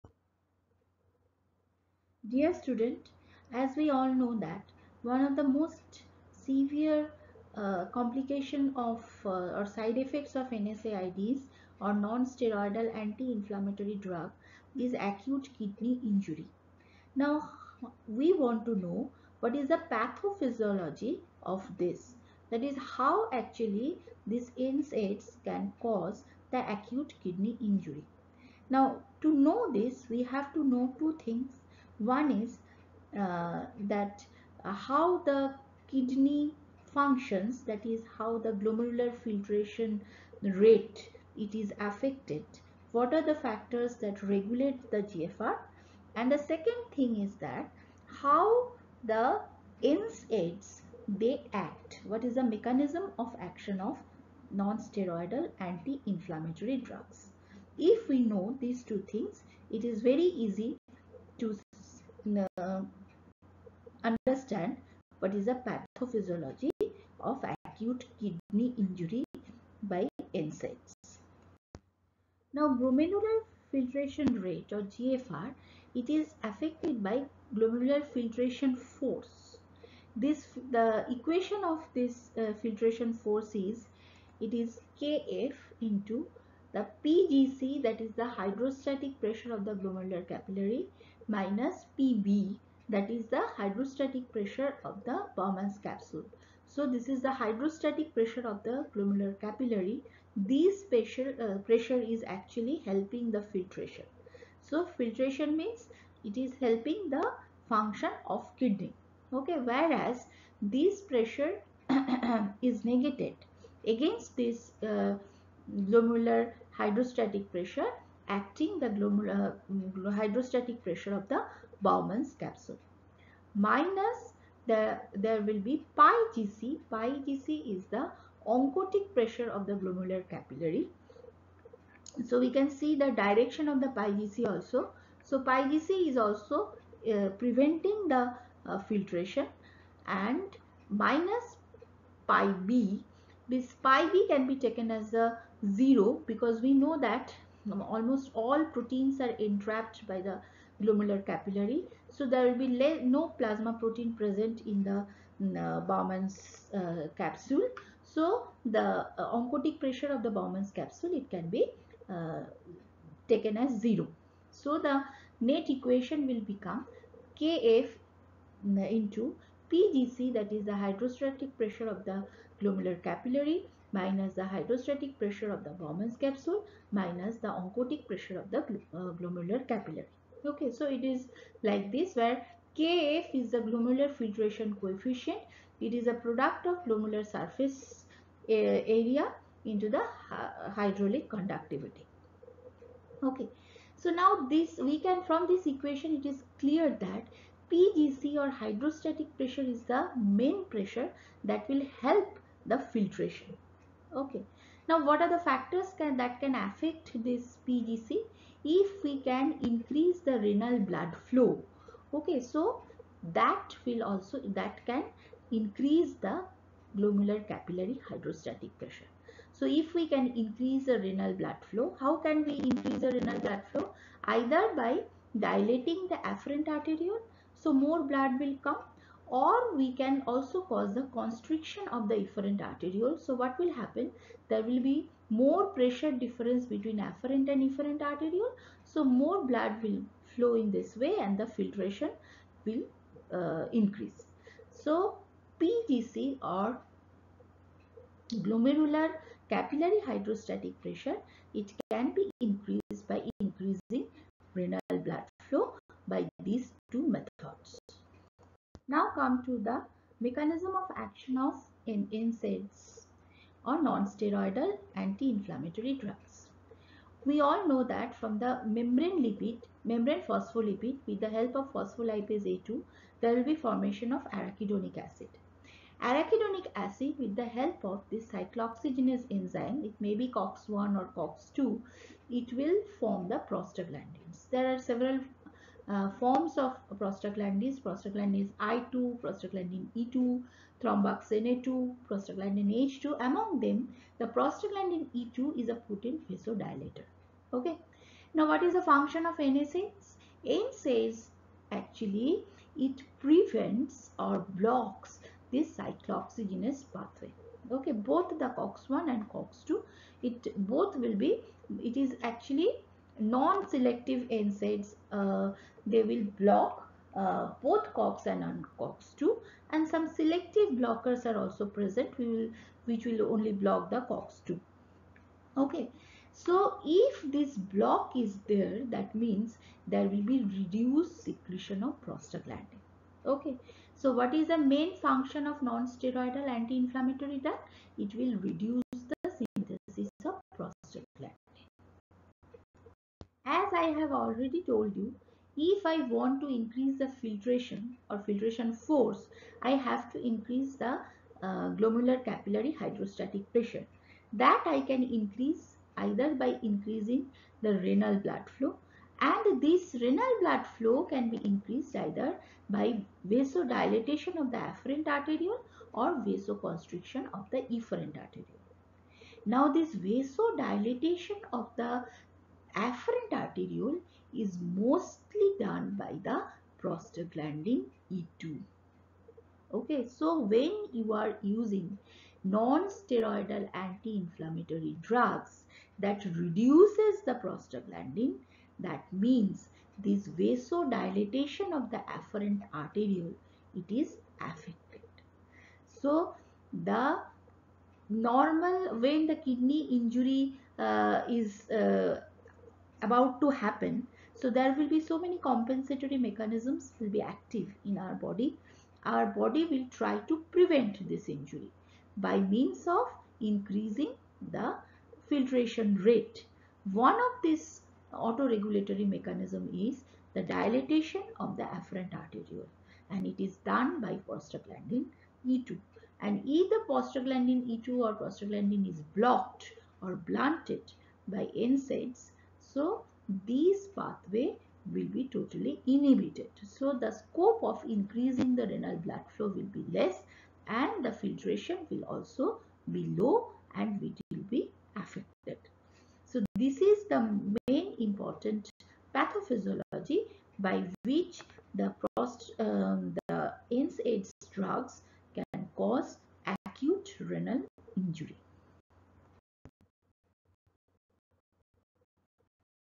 Acute kidney injury can be caused by NSAIDs
Side effect of NSAIDs, Acute kidney injury, prostaglandin, MBBS, Physiology, NEET PG